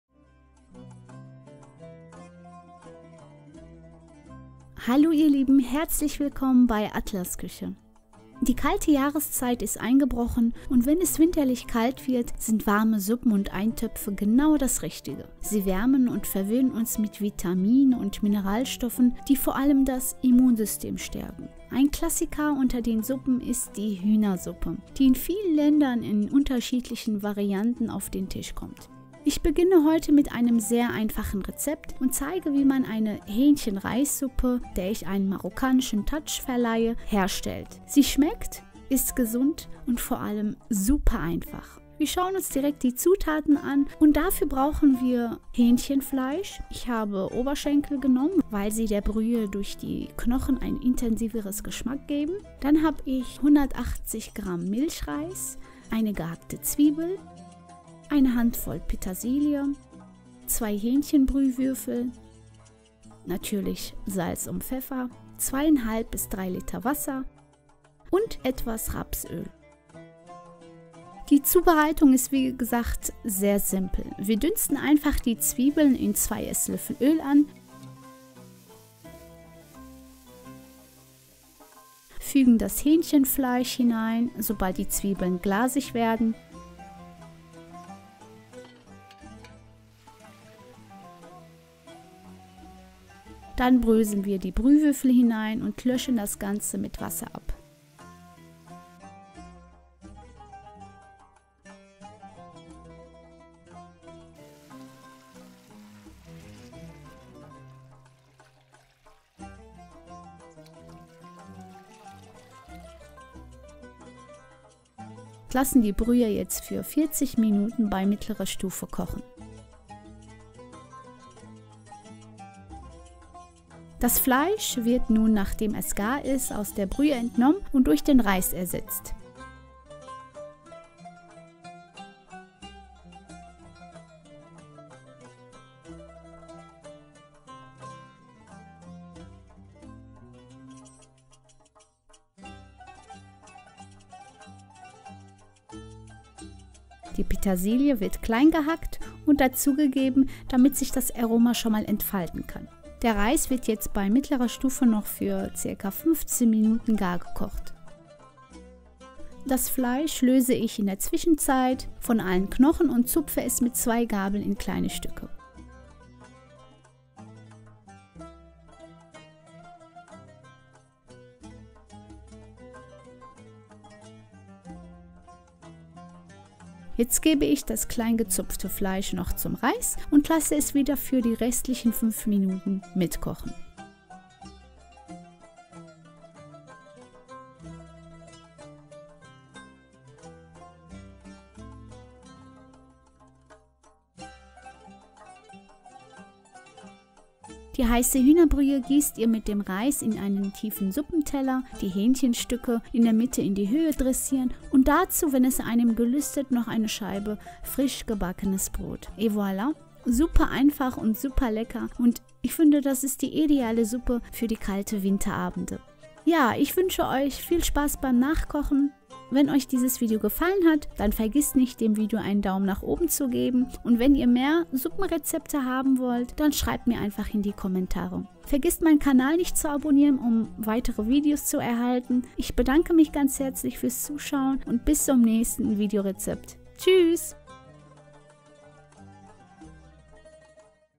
Hähnchen-Reis Suppe / Hühnersuppe
Herzlich Willkommen und schön das ihr zu mir gefunden habt.
Hühnersuppen sind ideal zu dieser kalten Jahreszeit, in diesem Video bereite ich eine super leckere und einfache Hühner Suppe zu.

Lasst gerne ein Abo da, wenn euch mein Kanal gefällt, Danke❤️